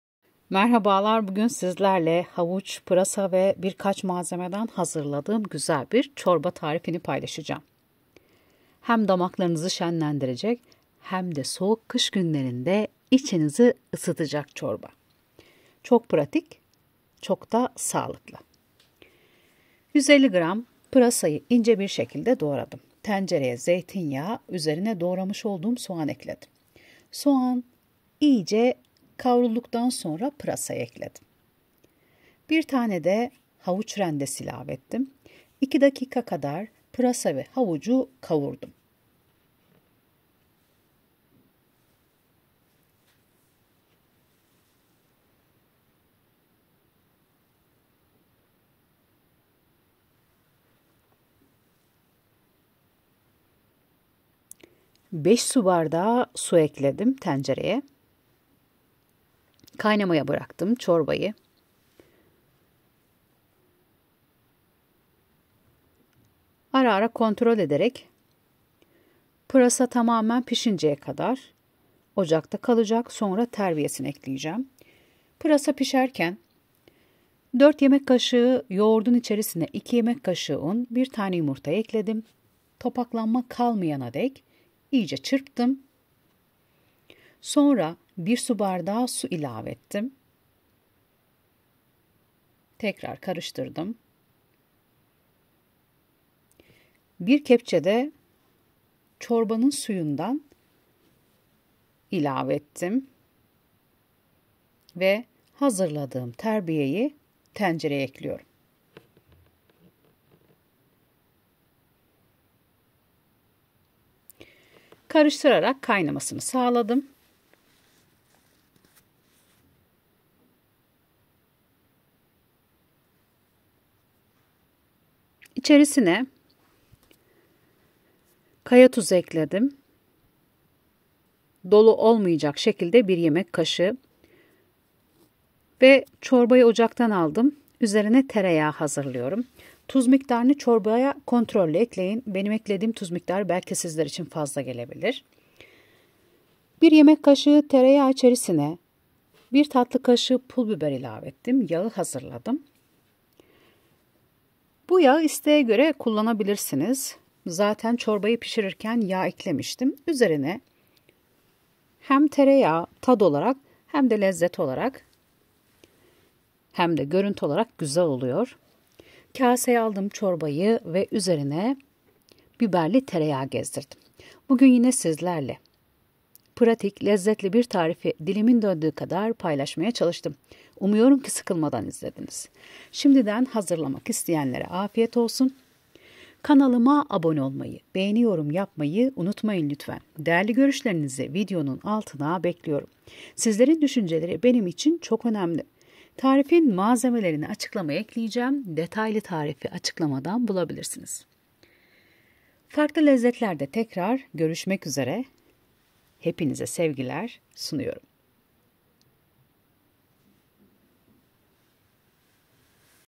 HEM ÇOK LEZZETLİ HEMDE ÇOK SAĞLIKLI KAŞIĞI ELİNİZDEN BIRAKMAYACAĞINIZ TARİF🤤
150: Gram  pırasa

50: Gram havuç

5: Su bardağı su bardak ölçüsü 300 ml

1: Kahve fincanı zeytinyağı

TERBİYESİ İÇİN

4: Yemek kaşığı yoğurt

2: Yemek kaşığı un

1: Tane yumurta

İSTEĞE GÖRE ÜZERİNE

1: yemek kaşığı tereyağı

1: Tatlı kaşığı pul biber